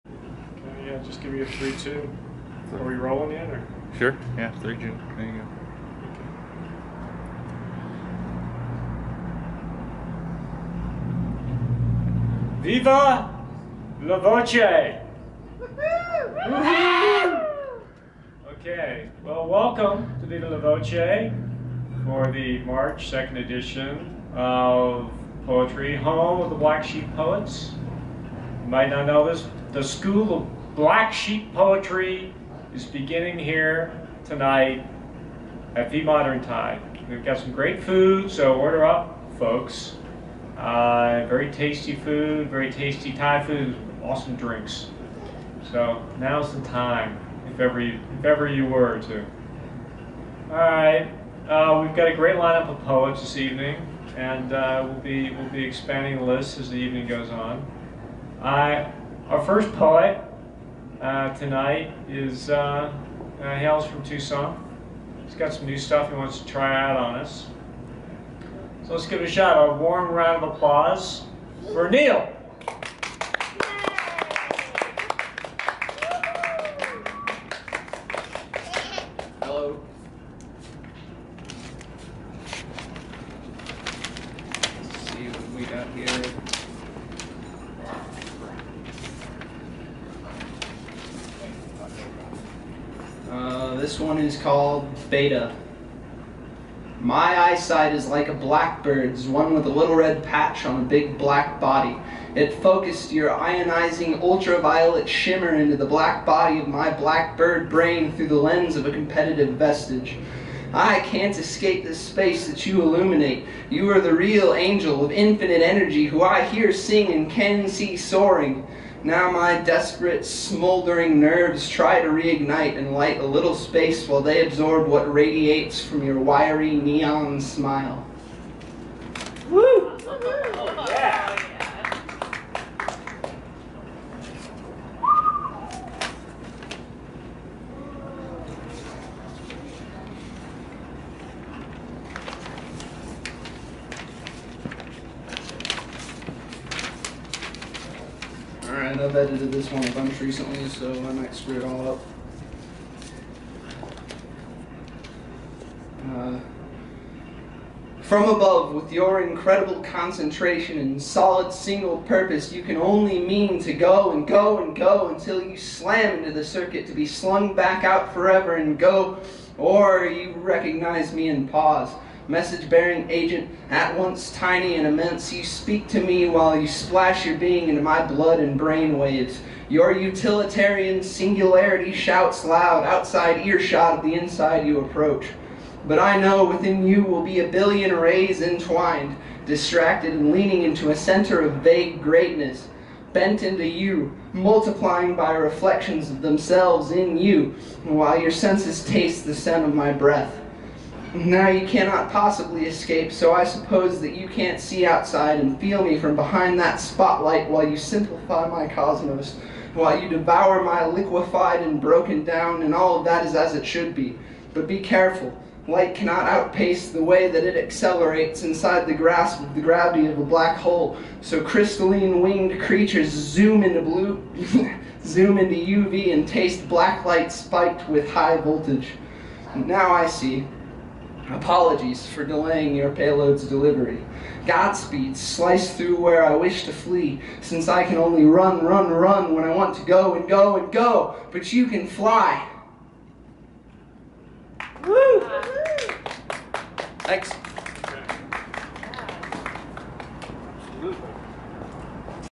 140319 Viva la Voce, Scott Stanley intros Neil Hillis 1 Beta 2 From Above
Viva la Voce, home of the Black Sheep Poets, based at V Modern Thai Restaurant. Scott Stanley, our Catalyst in Charge opens the evening and introduces Neil Hillis. Neil opens with 2 poems.